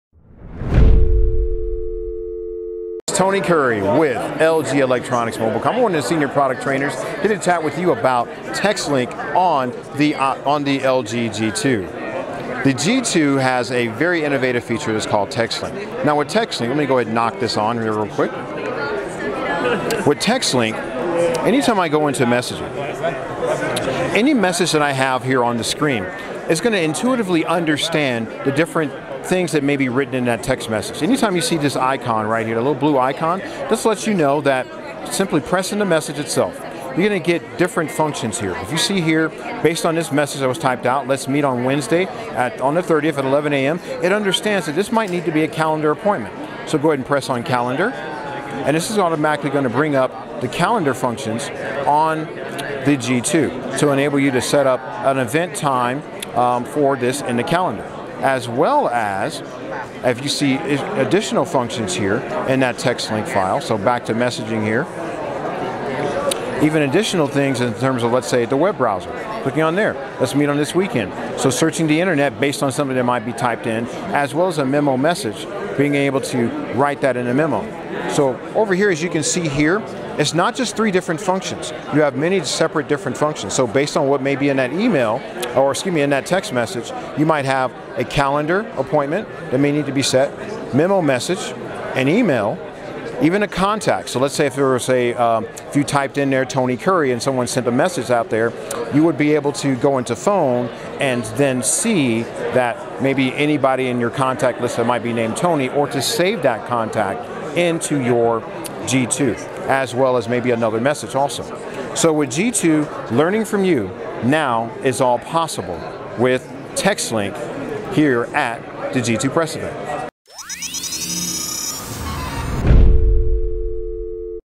LG G2 Text Link feature overview
PhoneArena presents an overview video of the LG G2 Text Link feature.